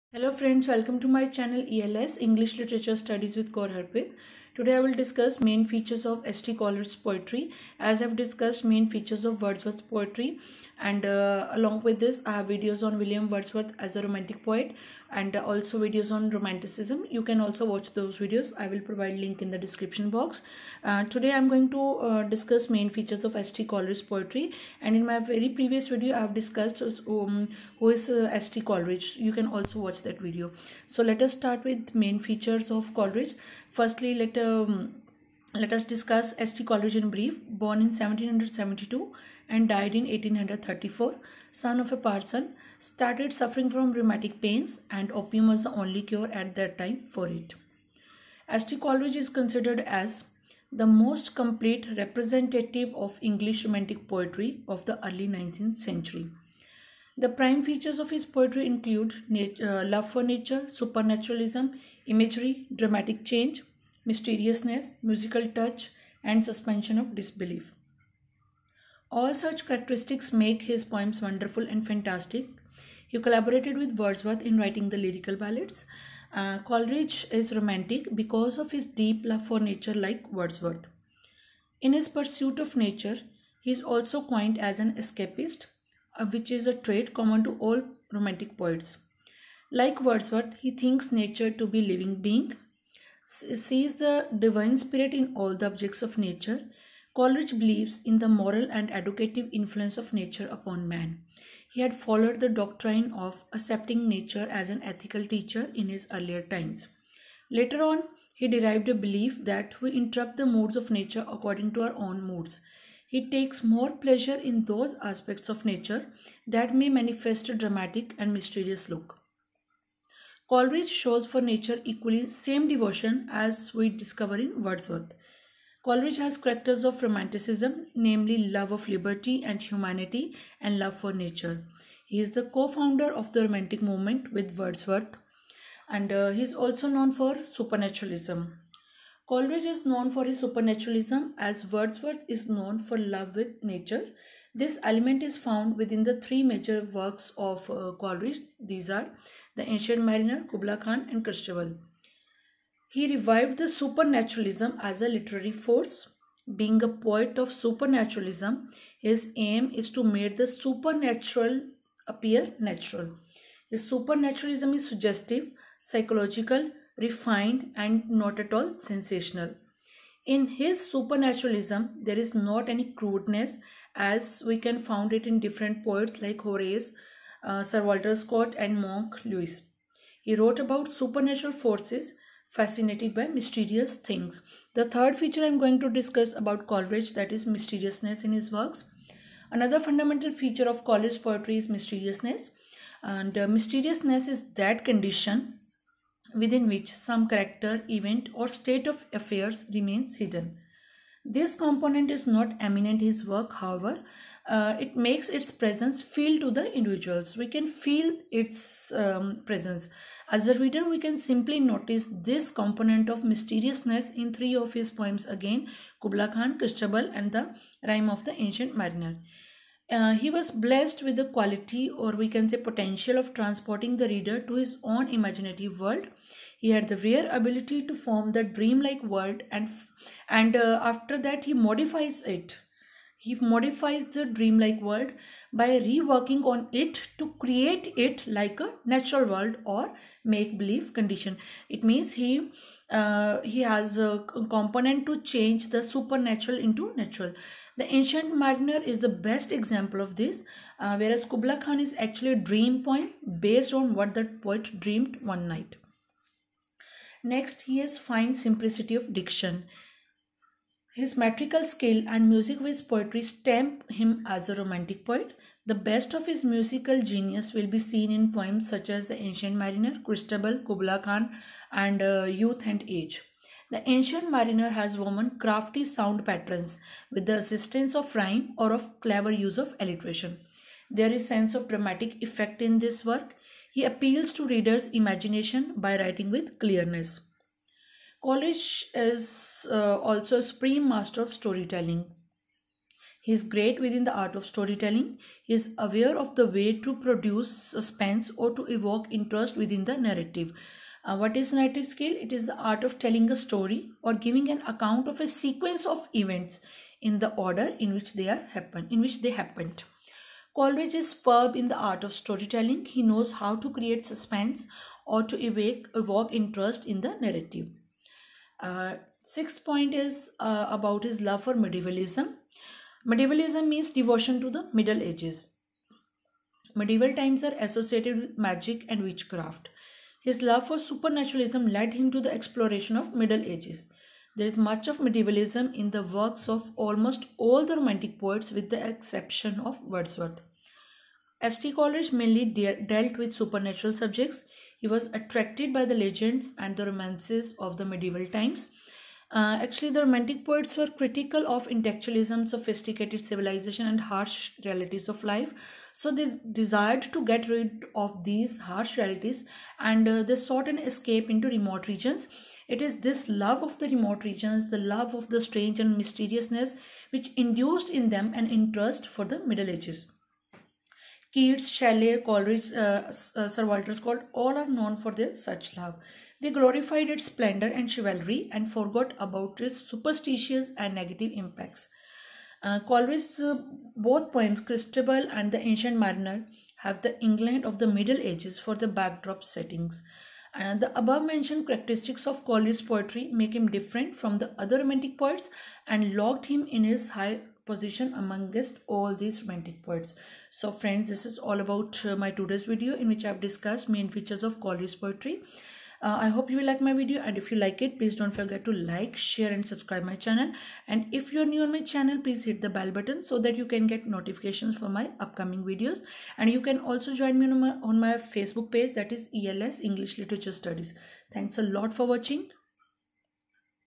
S.T. Coleridge / Features of poetry of S.T. Coleridge
Hello Friends Today's video is about main features of Coleridge's poetry. You can also check videos related to features of Wordsworth' poetry.